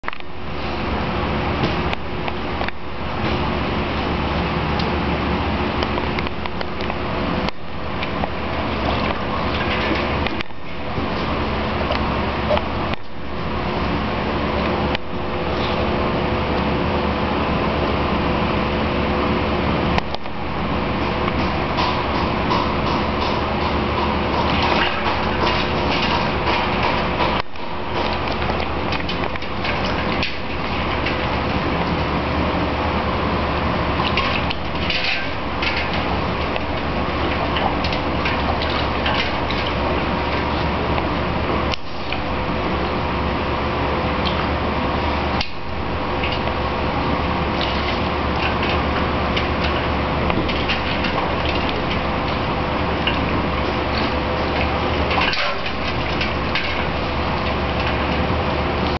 Great Dane snorkles and bobs for ice
My Great Dane Mia likes to snorkle and bob for ice!!